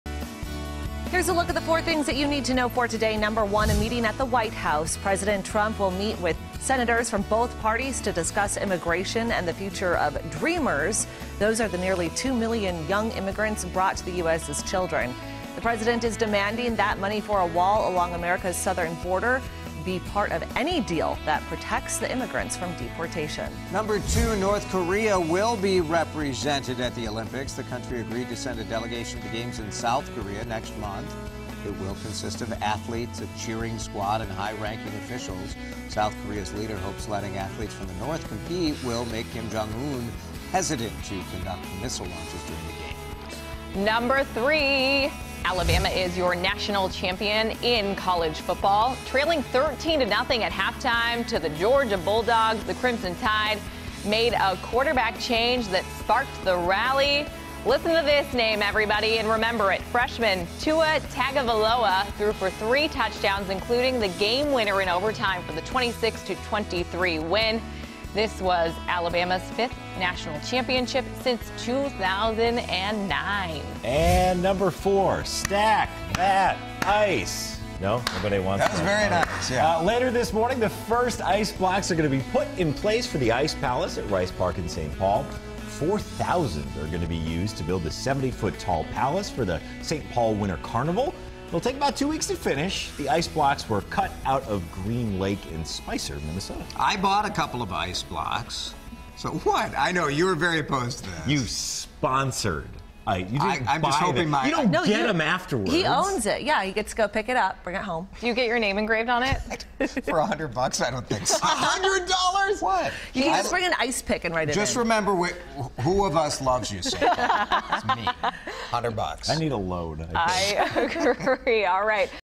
4 Things To Know From Jan. 9, 2017
They include: POTUS & DACA, North Korea represented at Olympics, Alabama wins championship and more. (2:02)
WCCO Mid-Morning – Jan. 9, 2018